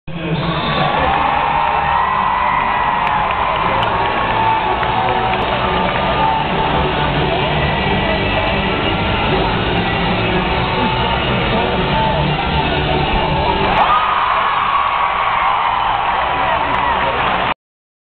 Morgan Dennis - Vault - Bama / Auburn - 26 Feb 2009
Video of the University of Alabama's Morgan Dennis during the annual "Power of Pink Meet" between the University of Alabama and Auburn University.  Morgan Dennis scored a 9.950 during Friday's (26 February 2009) wooping of Auburn in Coleman Coliseum in Tuscaloosa.  This was a night of season highs, when Morgan scored her highest in each event.